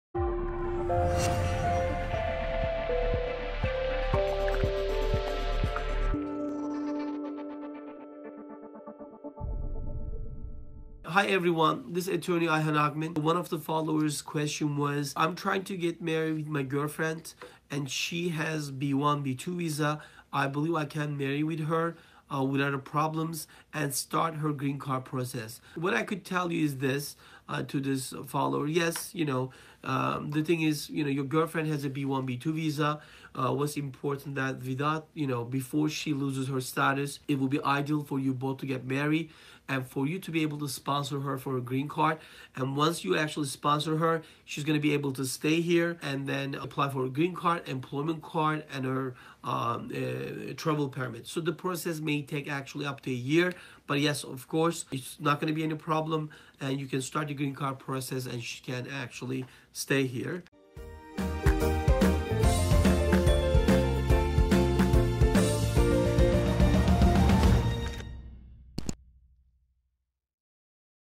Can I Marry With My Girlfriend Without Problems And Start The Green Card Process ?
Question: I am trying to marrie my girlfriend and she has a b1b2 visa. I believe can I marry with her without problems and start the green card process ?